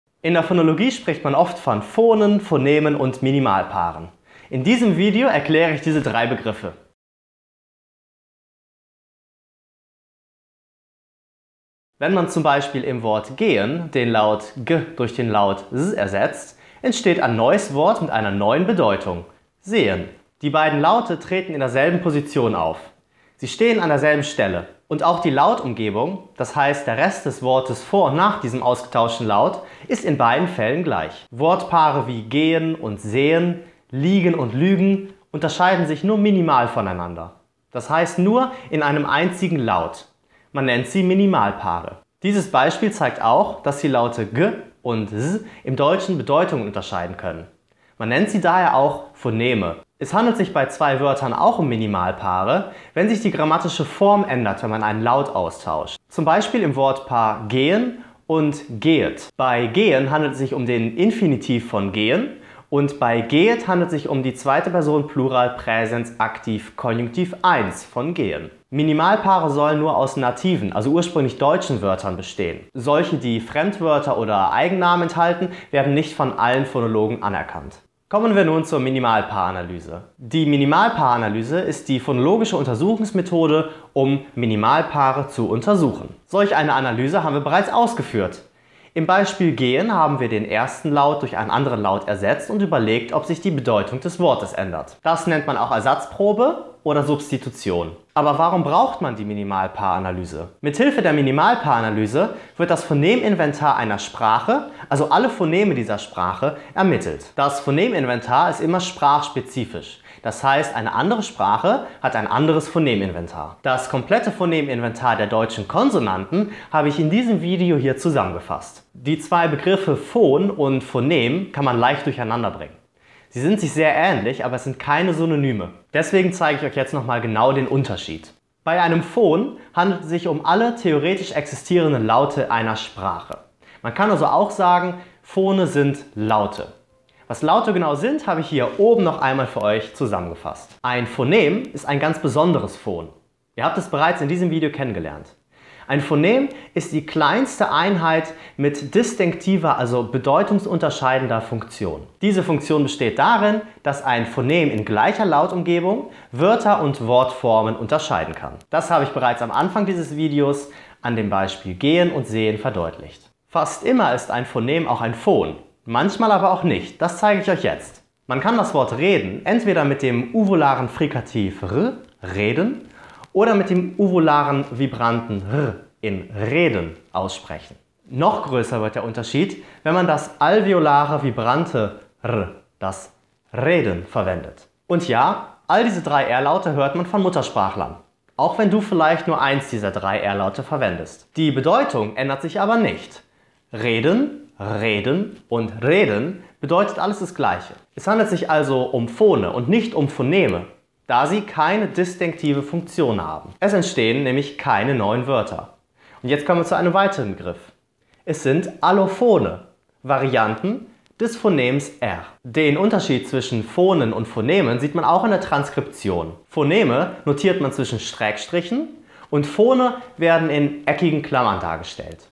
Phone, Phoneme, Minimalpaare und die Minimalpaaranalyse - Grundwissen Phonetik & Phonologie
In der Phonologie spricht man oft von Phonen, Phonemen und Minimalpaaren. In diesem Video erkläre ich diese drei Begriffe.

Quellen:
Šileikaitė-kaishauri, D. (2015). Einführung in die Phonetik und Phonologie des Deutschen.

Dieling, H., & Hirschfeld, U. (2000). Phonetik lehren und lernen. München, Deutschland: Langenscheidt.

Was sind Minimalpaare?
Wenn man z.B. im Wort gehen den Laut [g] durch den Laut [z] ersetzt, entsteht ein Wort mit einer anderen Bedeutung: sehen [ˈzeːən]. Die beiden Laute treten in derselben Position auf, also sie stehen an derselben Stelle und die Lautumgebung, d.h. der Rest des Wortes vor und nach diesem ausgetauschten Laut, ist in beiden Fällen gleich. Wortpaare wie gehen : sehen oder legen : lügen unterscheiden sich somit nur minimal voneinander. Das heißt nur durch einen einzigen Laut. Man nennt dies Minimalpaare. Dieses Beispiel zeigt auch, dass /g/ und /z/ im Deutschen Bedeutungen unterscheiden können. Diese Laute sind somit Phoneme des Deutschen.

Es handelt sich bei zwei Wörtern auch um Minimalpaare, wenn sich die grammatische Form ändern, wenn man einen Laut austauscht. Zum Beispiel im Wortpaar [ˈgeːən] und [ˈgeːət]. Bei gehen handelt es sich um den Infinitiv von gehen und bei gehet um die 2. Person,. Plural, Präsens, Aktiv, Konjunktiv I.
Minimalpaare sollen nur aus nativen (ursprünglich deutschen) Wörtern bestehen. Solche, die Fremdwörter oder Eigennamen enthalten, werden nicht von allen Phonologen anerkannt.
Wie funktioniert die Minimalpaaranalyse?

Die Minimalpaaranalyse ist die phonologische Untersuchungsmethode, bei der man Minimalpaare untersucht. Wir haben wir bereits ausgeführt, indem wir im Wort gehen einen Laut durch einen andern ersetzt und überlegt haben, ob sich die Bedeutung des Wortes geändert hat. Dies nennt man auch die Ersatzprobe oder Substitution.

Aber warum braucht man die Minimalpaaranalyse?
Mithilfe der Minimalpaaranalyse wird das Phoneminventar einer Sprache, d.h. alle Phoneme dieser Sprache ermittelt. Das Phoneminventar ist immer sprachspezifisch, es gilt nur für diese konkrete Sprache. In einer anderen Sprache gibt es ein anderes Phoneminventar. Das komplette Phoneminventar der deutschen Konsonanten habe ich diesem Video zusammengefasst.
Der Unterschied zwischen Phon und Phonem.

Die Begriffe Phon (also Laut) und Phonem bringt man leicht durcheinander. Deswegen möchte ich noch einmal deutlich den Unterschied darstellen. Es sind nämlich keine Synonyme.
Bei einem Phon handelt es sich um alle theoretisch existierenden Laute einer Sprache mit einer bestimmten Funktion. Also etwas vereinfacht könnte man auch sagen: Phone sind die Laute einer Sprache. Hier oben über den Link kommst du direkt zu meinem Video „was sind Laute“.

Ein Phonem aber ist ein ganz besonderes Phon. Ihr habt es in diesem Video kennengelernt. Es ist die kleinste lautliche Einheit mit distinktiver oder eben bedeutungsunterscheidender Funktion. Diese Funktion besteht darin, dass ein Phonem in der gleichen Lautumgebung Wörter und Wortformen unterscheiden kann. Das habe ich am Anfang dieses Videos anhand des Minimalpaars gehen und sehen verdeutlicht.

Fast immer ist ein Phonem auch ein Phon, manchmal aber auch nicht. Ich habe hier ein Beispiel, dass das verdeutlicht.
Man kann das Wort reden entweder mit einem uvularen Frikativ aussprechen, also reden oder mit einem uvularen Vibranten: reden. Noch größer wird der Unterschied, wenn man das alveolare Vibrante R verwendet: reden. Und ja, all diese drei R Laute werden von Muttersprachlern in Deutschland verwendet. Auch wenn du vielleicht nur eines dieser drei R Laute verwendest. Die Bedeutung ändert sich nicht: reden, reden und reden bedeutet dasselbe. Es handelt sich um Phone, es sind aber keine Phoneme, da sie keine distinktive Funktion haben. Es entstehen keine neuen Wörter. Es sind, und jetzt kommt wieder ein neuer Begriff, Allophone, also zwei weitere Varianten des Phonems R.

Der Unterschied zwischen Phonen und Phonemen kommt auch in der Transkription zum Ausdruck:

Die in einer Sprache theoretisch existierenden Phoneme notiert man bei der phonologischen Transkription zwischen Schrägstrichen, z.B. /a:/.

Die Phone, die beim Sprechen als Realisierungen von Phonemen artikuliert werden, stehen bei der phonetischen Transkription in eckigen Klammern, z.B. [aː].